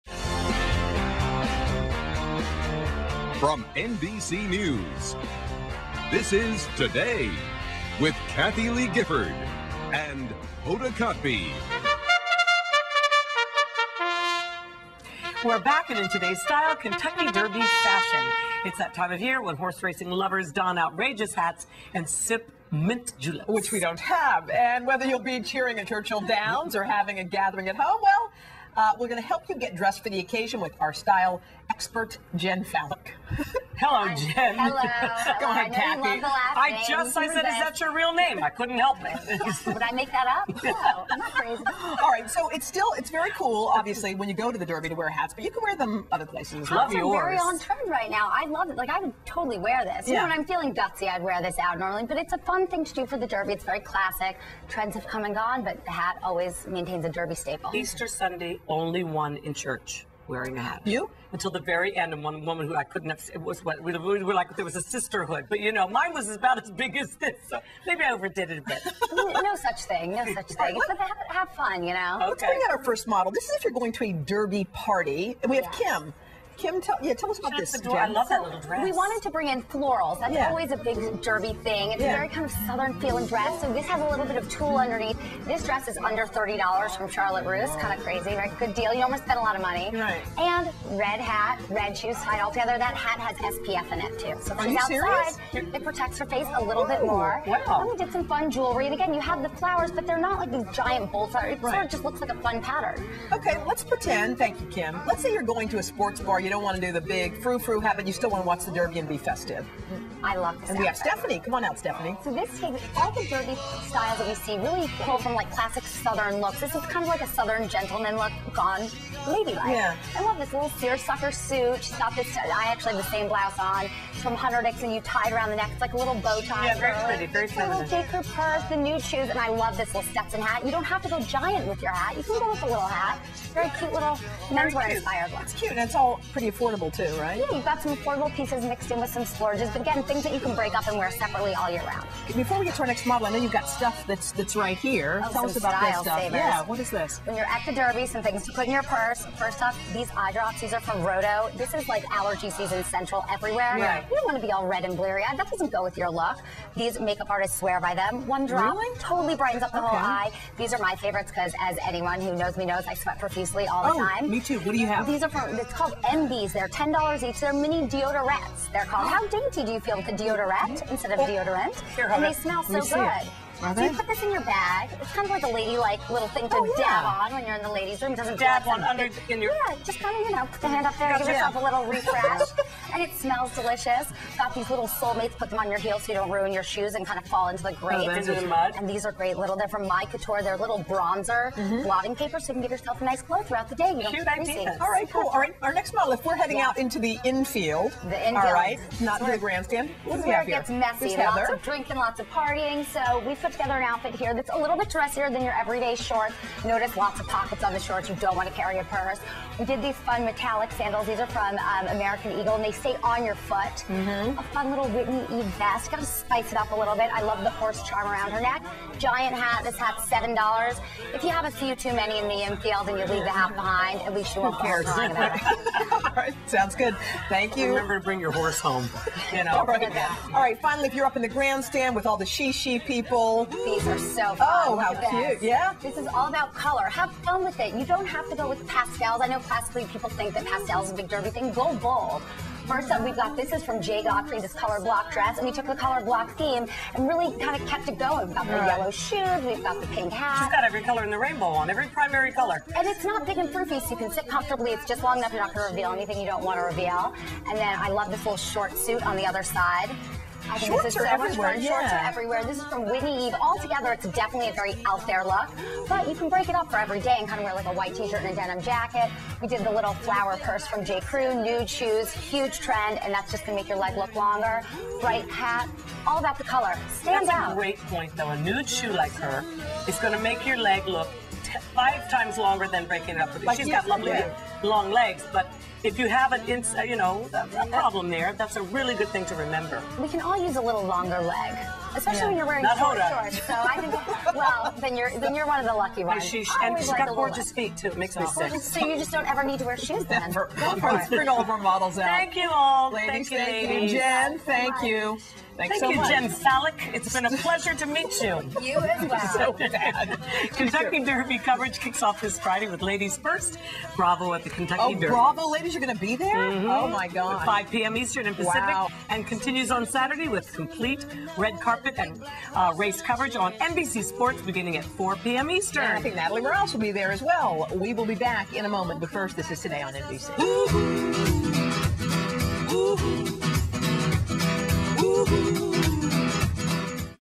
How to Dress for the Kentucky Derby
With Kathie Lee Gifford, Hoda Kotb, and Liliana Vasquez.